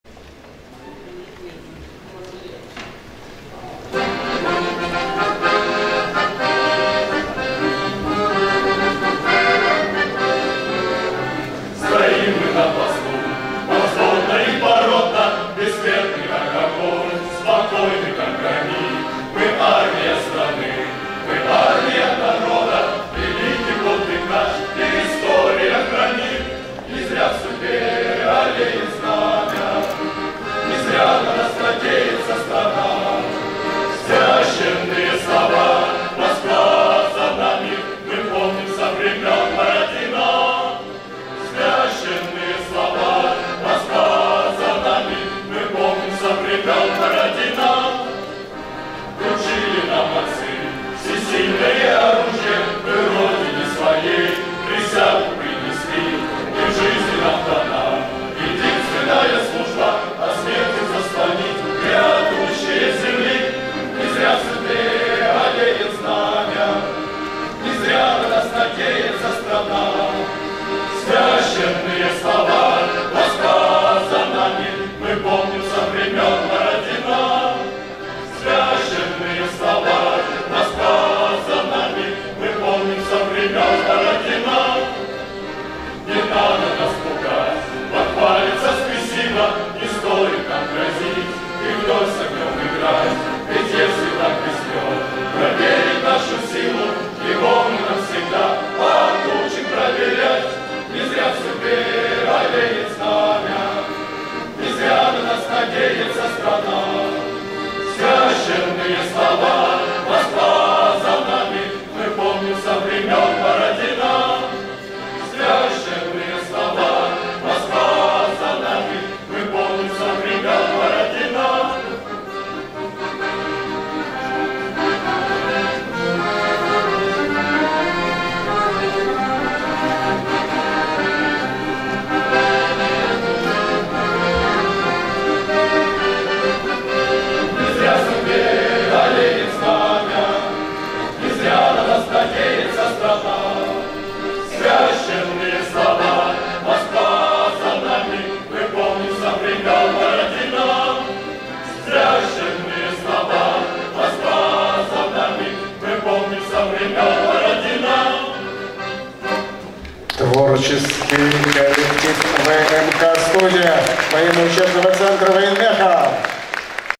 2019 11 19 7
19 ноября 2019 г. Приглашаем Вас на торжественное мероприятие, посвященное дню ракетных войск и артиллерии и празднованию 100-летия М.Т. Калашникова